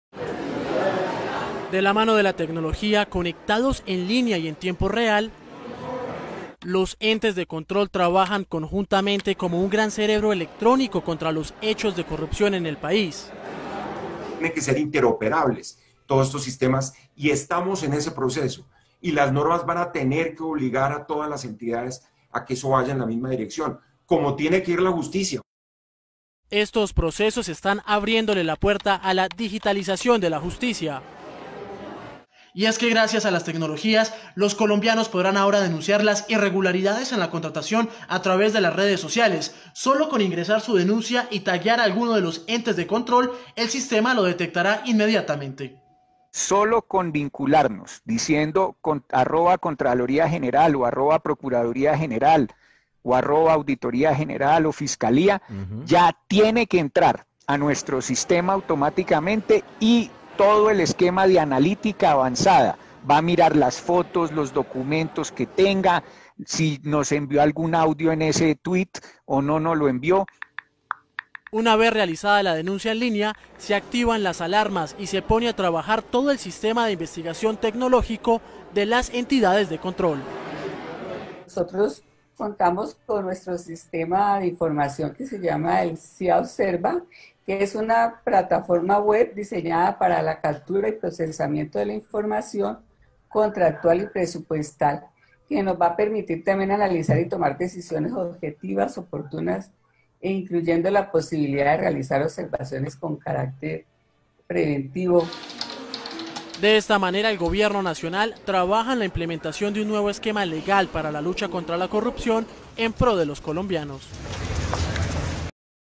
En el foro tecnologia para la transparencia los entes de control hablaron sobre la corrupciòn
VÍA: LE CONTAMOS